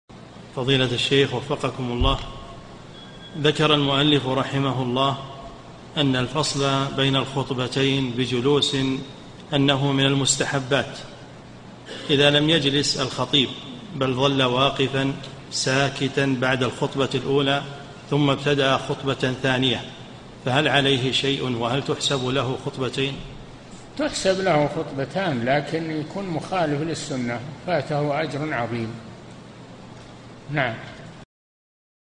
[850 -3022] إذا لم يجلس الخطيب بعد الخطبة الأولى بل ظل واقفا ساكتا ثم ابتدأ خطبة ثانية هل عليه شيء؟
-"[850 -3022] السؤال: ذكر المؤلف رحمه الله أن الفصل بين الخطبتين بجلوسٍ أنه من المستحبات، إذا لم يجلس الخطيب بل ظل واقفاً ساكتاً بعد الخطبة الأولى ثم ابتدأ خطبةً ثانية، فهل عليه شيء وهل تحسب له خطبتين؟
الجَوَابُ: تحسب له خطبتان لكن يكون مخالف للسنة فاته أجْر عظيم، نعم.
للاشتراك في قناة الشيخ صالح الفوزان

للاستفسار والإقتراحات والدعم
رئيس الفريق
لمشروع كبار العلماء